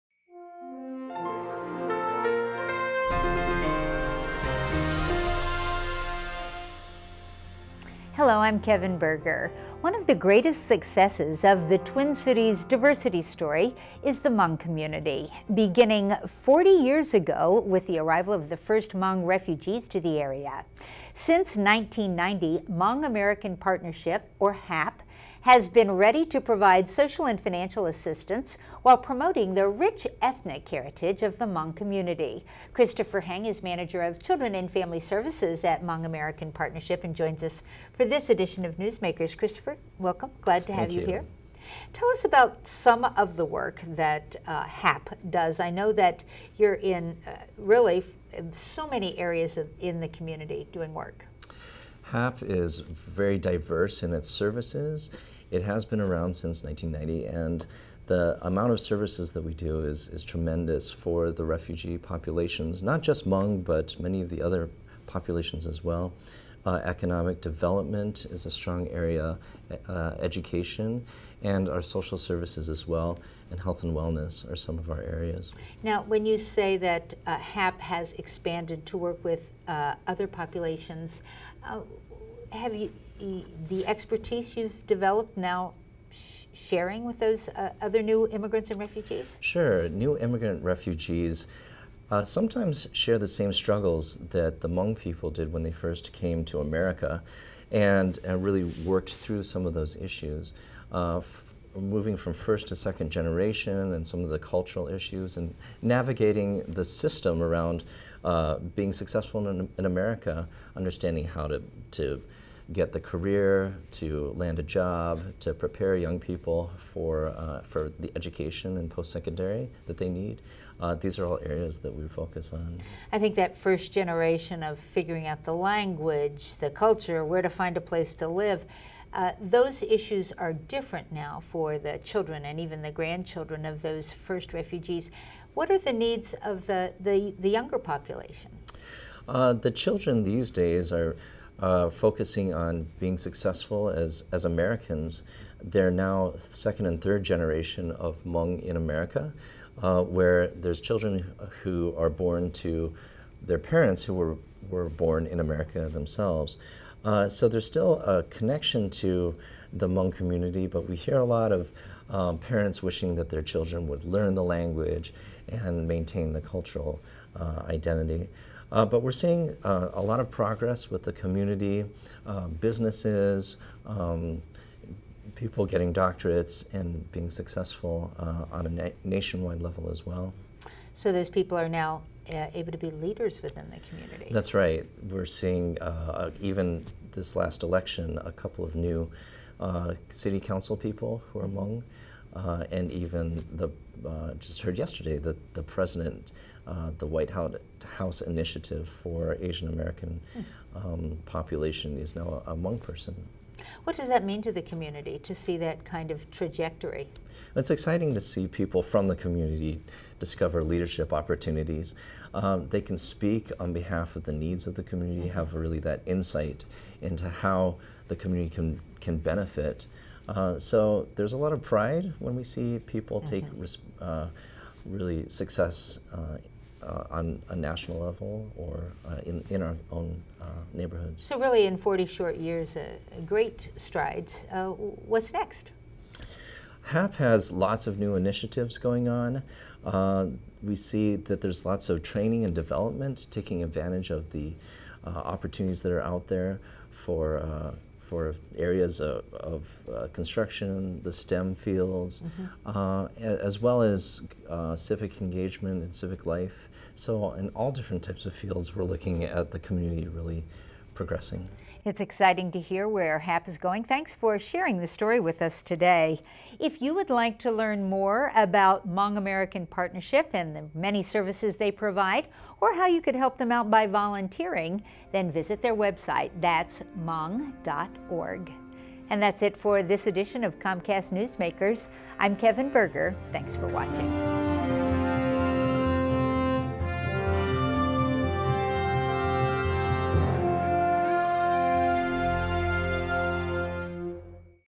Hmong American Partnership
Forty years ago, the first Hmong refugees started arriving in the Twin Cities and since 1990 the Hmong American Partnership has been there to offer services and support.  Learn more on this edition of Comcast Newsmakers.